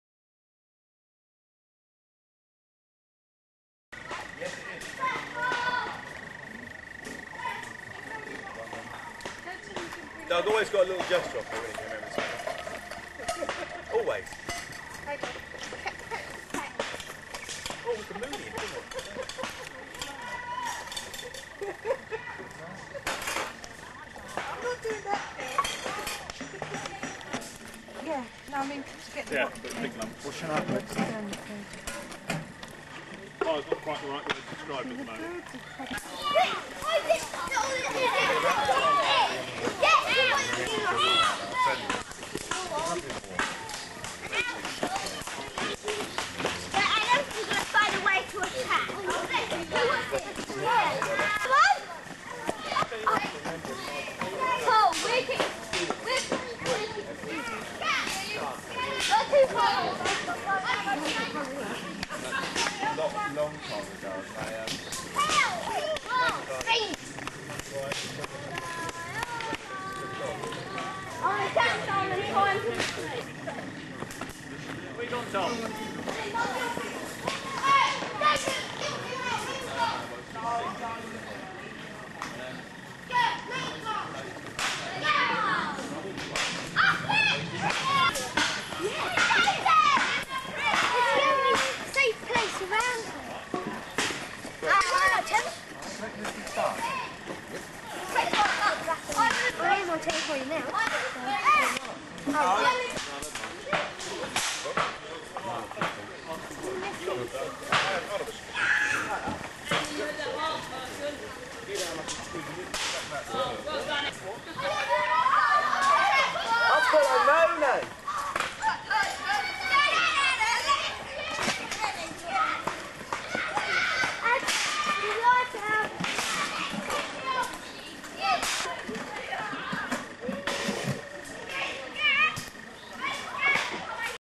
FandS92
3rd Bromley Scout Group Father and Son camp 1992. This was at Downe Camp site. The theme of the camp was "China".